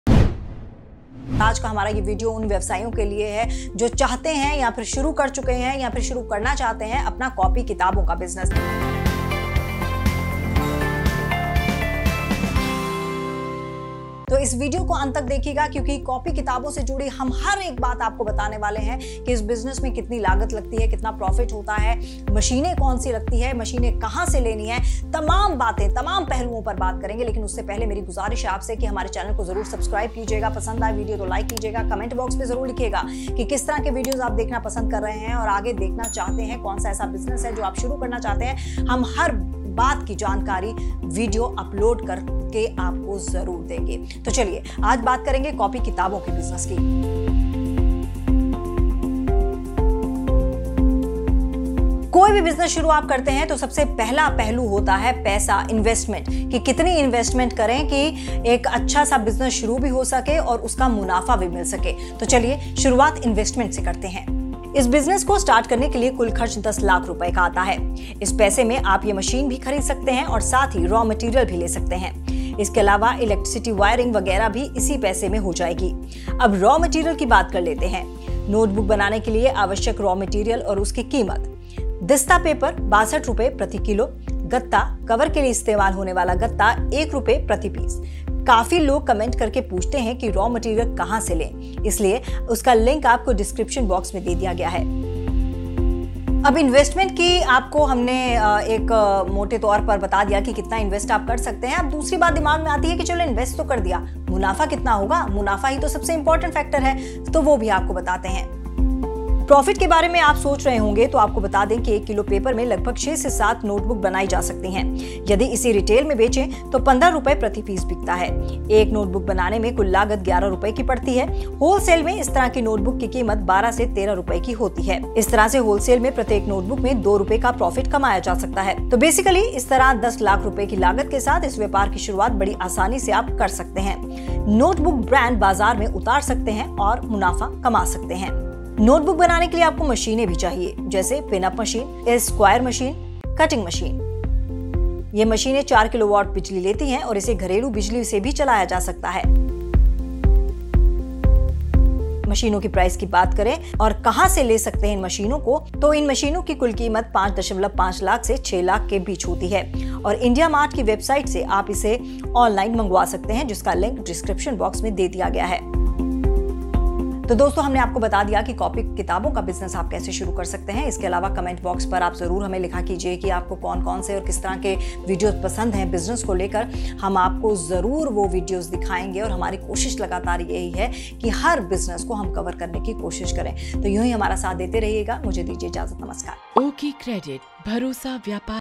कॉपी बनाने का व्यापार कैसे करें | Notebook Manufacturing Business in Hindi
अगर आप कॉपी बनाने का बिज़नेस करने के बारे में सोच रहे हैं तो ये वीडियो आपके लिए है, क्युंकि आज के इस वीडियो में हम आपको इस बिज़नेस से जुड़ी सभी जानकारियां देंगे जैसे कि रॉ मैटेरियल क्या क्या चाहिए, मशीन कहां से मिलेगी, कितने की मिलेगी, जगह कितनी चाहिए होगी आदि।

कच्चा माल (रॉ मैटेरियल) खरीदने के लिए नीचे दिए गए लिंक पर क्लिक करें

यदि आप कॉपी बनाने के बिज़नेस के लिए मशीनें ऑनलाइन मंगवाना चाहते हैं तो नीचे दिए गए लिंक की मदद ले सकते हैं।